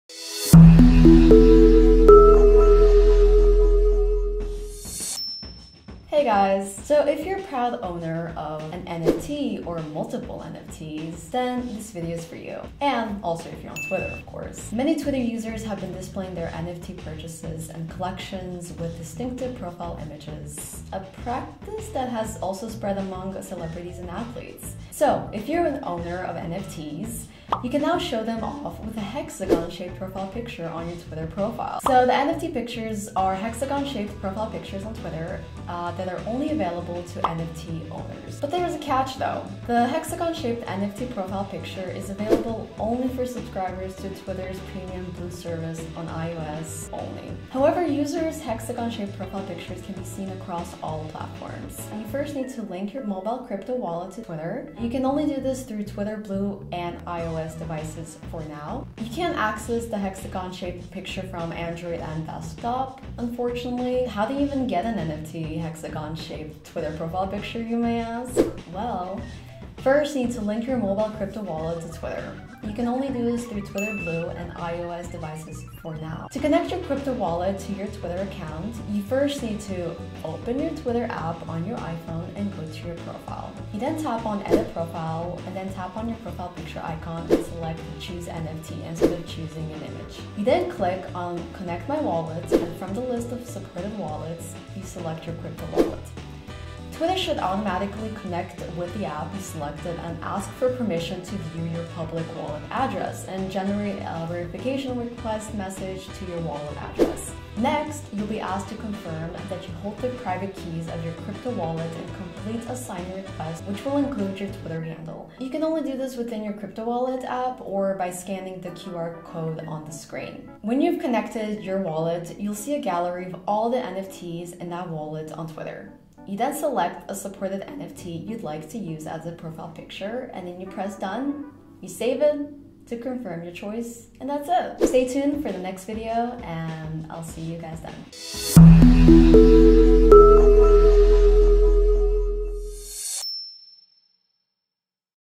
How to Set Your NFT as Profile Picture on Twitter
Twitter has been a front where cryptocurrency investors have been flaunting their big crypto purchases. Many Twitter users have been promoting their NFT purchases and collections with unique profile pictures. Watch this video to find out how to set your NFT as your profile picture on Twitter.